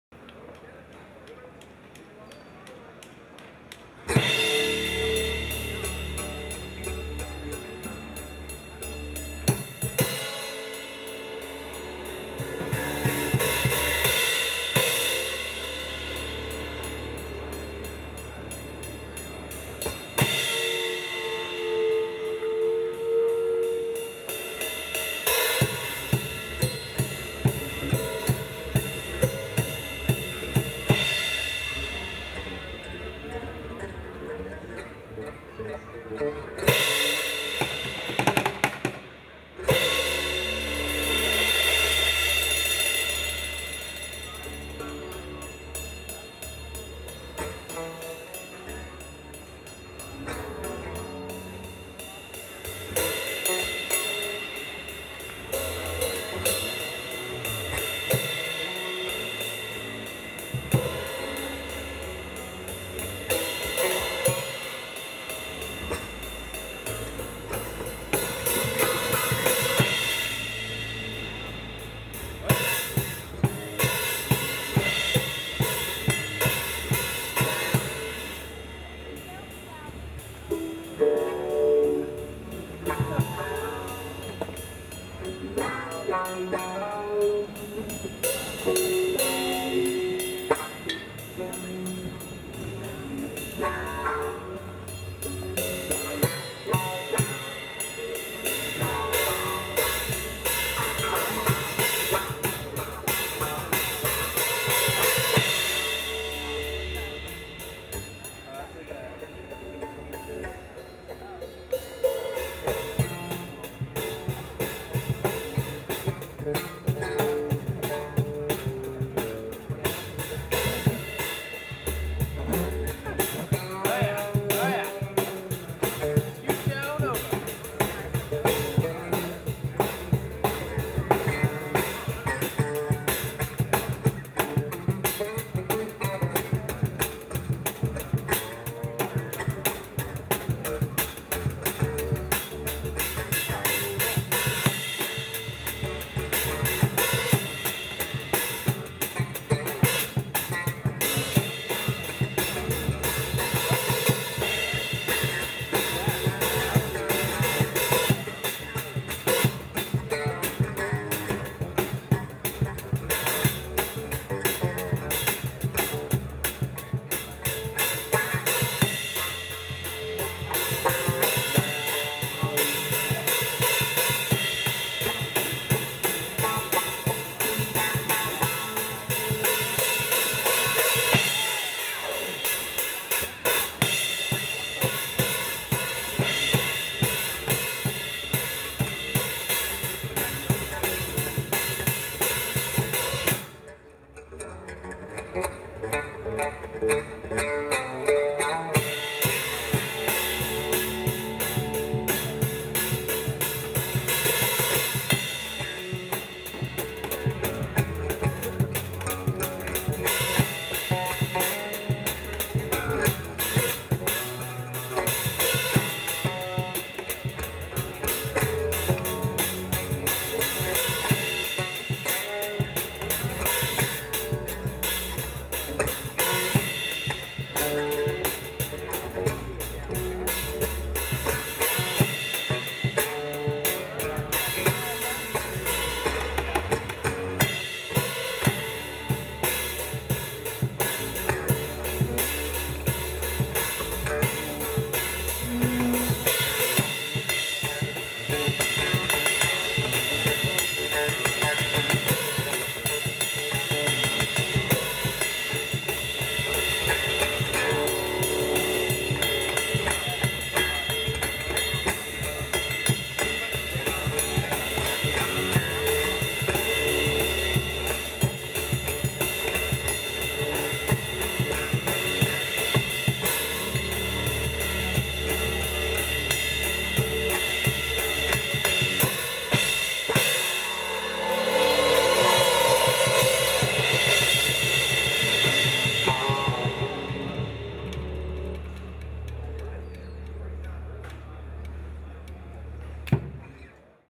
Entertainment Dean Peer
Dean Peer Performs at T.H.E. Show Newport 2013